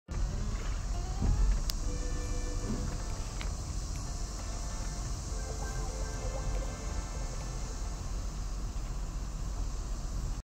🦓 Zebra 🦓 zebra lion country safari
Zebras enjoy meal . Lion Country Safari Loxahatchee Florida USA.
0:00 zebra in safari Loxahatchee Florida USA.